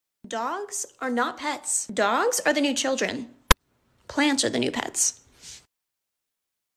Why Dogs are the New Children and Plants are the New Pets
Tanks fur watching today's reel, "Why Dogs are the New Children and Plants are the New
Pets." In this hilarious and heartwarming video, join me, the world's most proud dog mom of 4 Merle French Bulldogs, as I share my thoughts on the latest trend in pet ownership. Forget about calling dogs "pets" - they are now officially the new children! And if you're looking for a new pet, forget about cats or birds - plants are the new pets! Watch as I cuddle with my adorable pups and share some insights into the joys and challenges of raising dogs in today's world. Whether you're a lifelong dog lover or just looking for a good laugh, this video is sure to delight and entertain. So, grab some popcorn, sit back, and enjoy the show!

Sinzcerely,
The Merle Frenchies
💖I appreciate each and every one of my viewers and I love you guys so much!💖

© The Merle Frenchies Trending Instagram reels of 2023